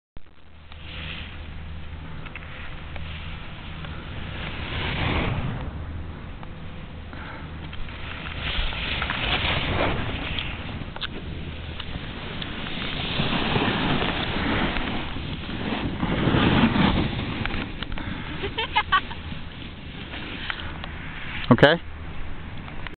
carrie doing snowskateboarding
the most beautiful extreme athlete takes the slopes, then marks her triumphant descent with a pump of the fisticuffs.